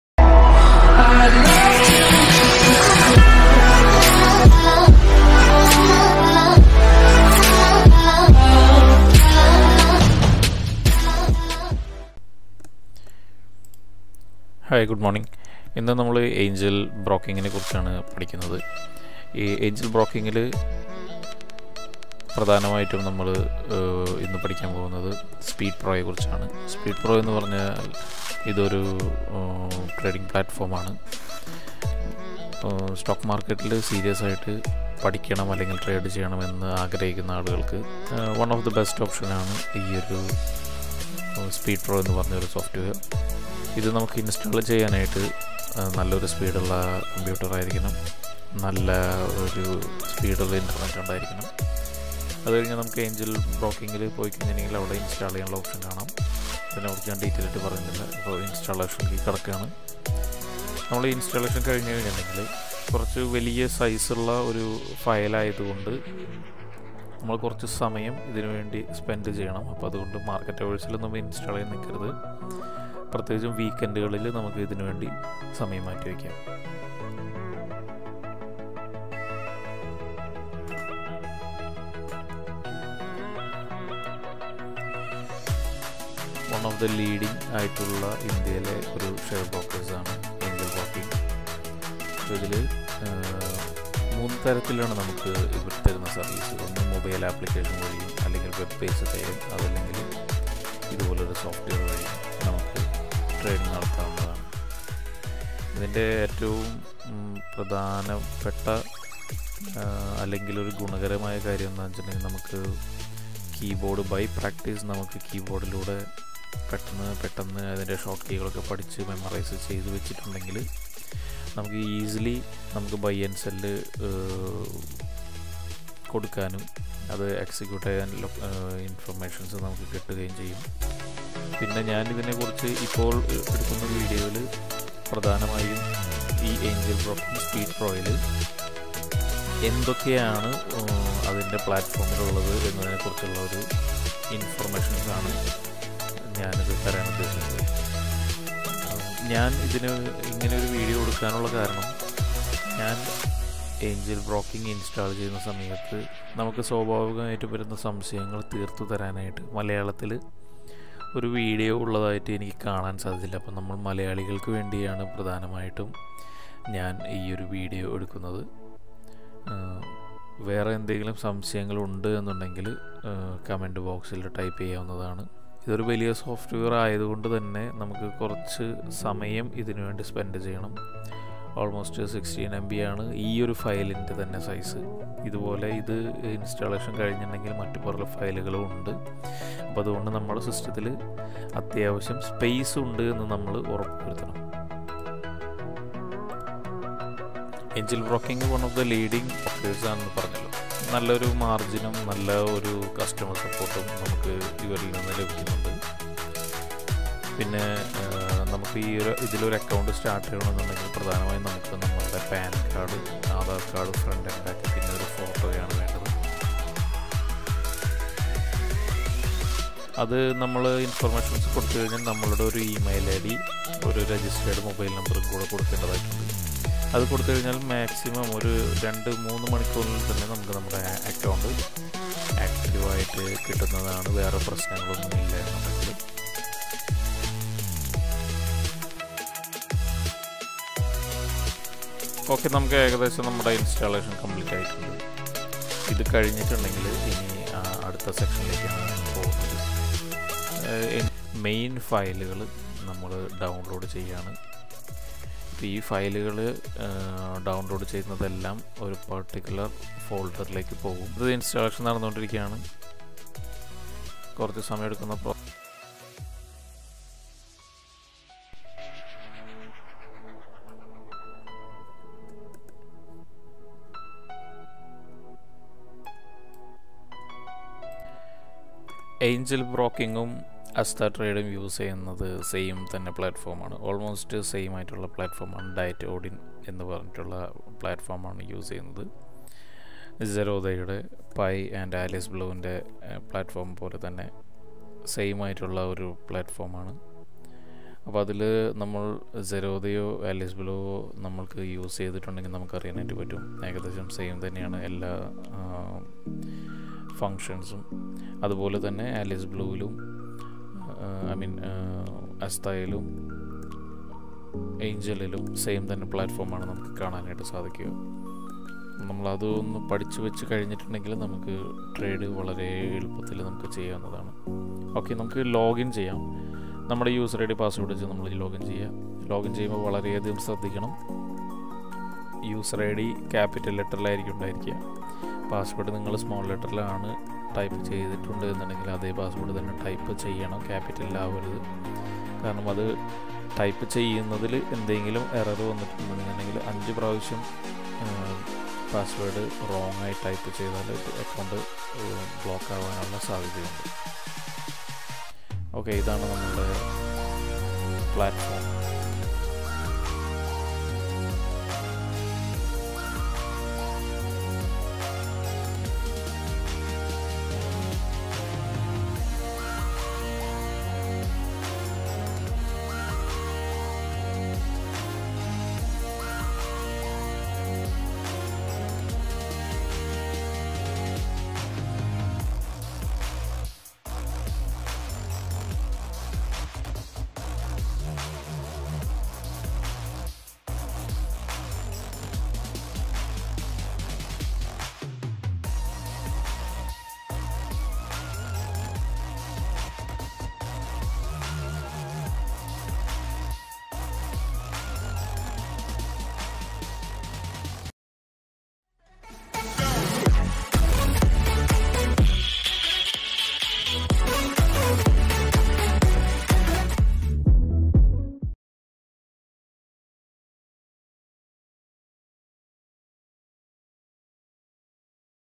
LEARN ANGEL BROKING SPEED PRO IN MALAYALAM
മലയാളികൾക്ക് വേണ്ടി

to learn the basics of angel broking speed pro trading platform